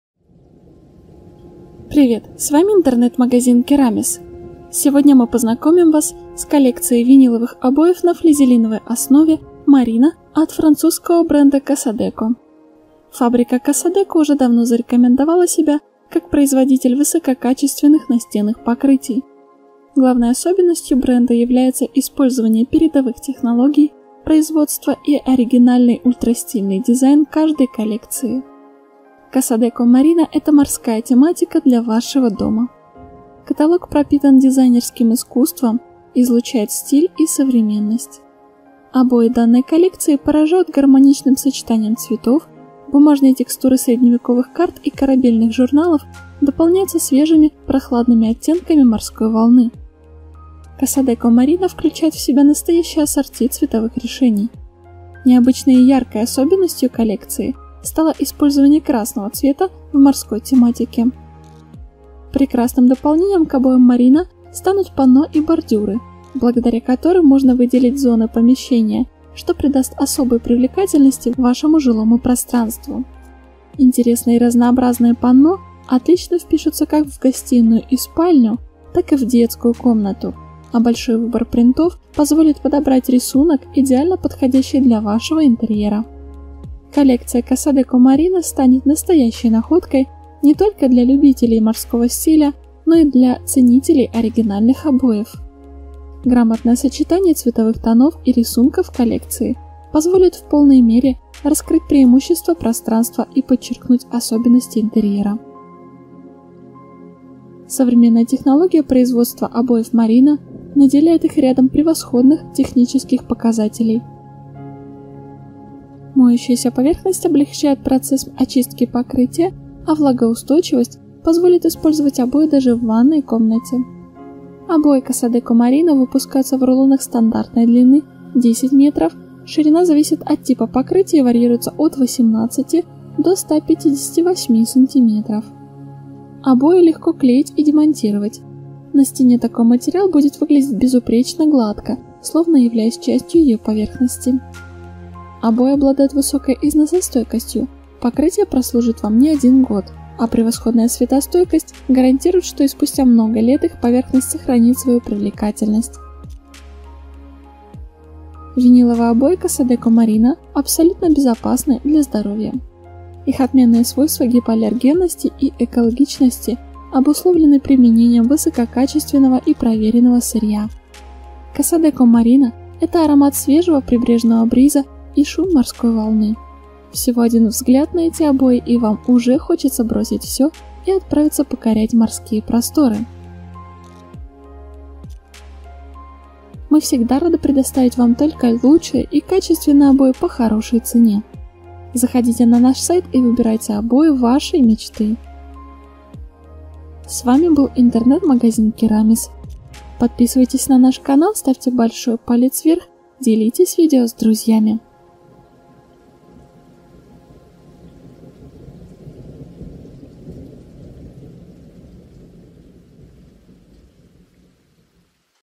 Морские акценты на ваших стенах с Casadeco Marina
Обои Casadeco Marina поражают гармоничным сочетанием цветов, бумажные текстуры средневековых карт и корабельных журналов дополняются свежими прохладными оттенками морской волны.
Каталог пропитан дизайнерским искусством, излучает стиль и современность.
Сasadeco Marina включает в себя настоящее ассорти цветовых решений. Необычной и яркой особенностью коллекции стало использование красного цвета в морской тематике.

Краткие характеристики:
Страна
Производитель
Коллекция: Marina
Ширина рулона: 18 см, 53 см, 110 см, 158 см
Длина рулона: 10,05 м.п.